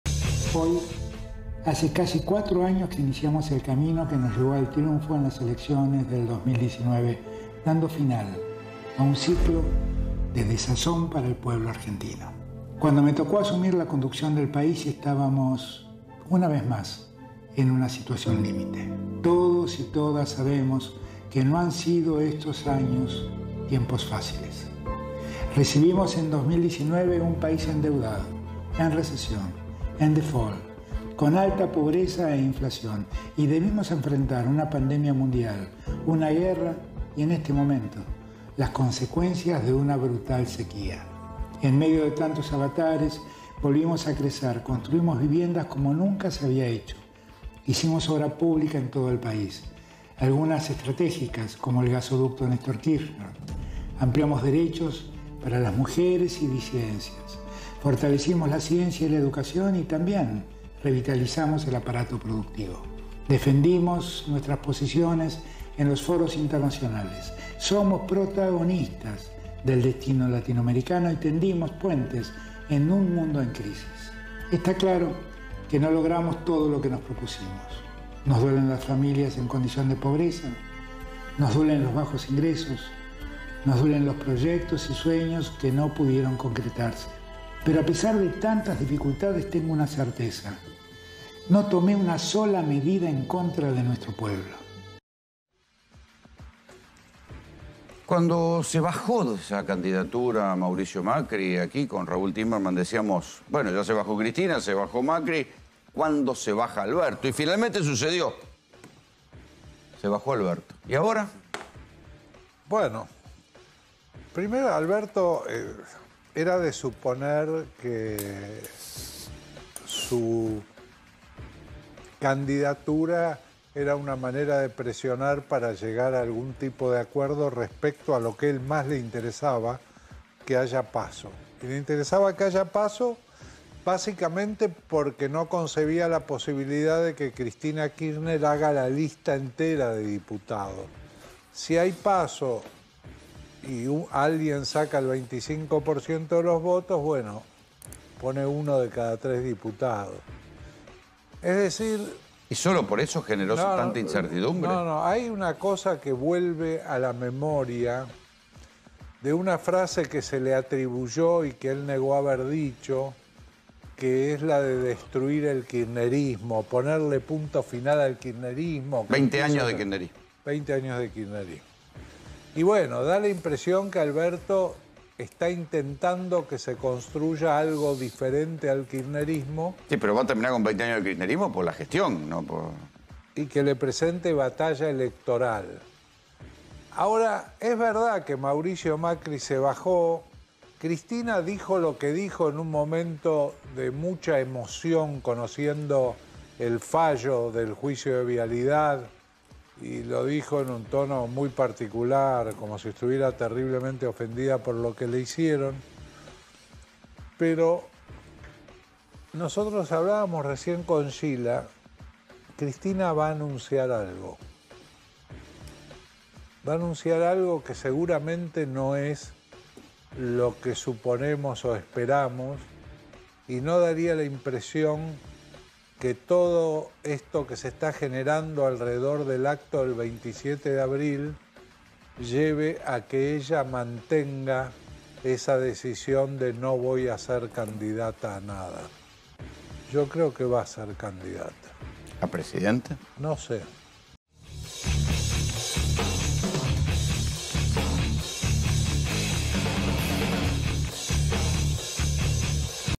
GPS | Se bajó ALBERTO, ¿Y ahora qué?
Se bajó Cristina Kirchner. Se bajó Mauricio Macri. Se bajó Alberto. ¿Y ahora?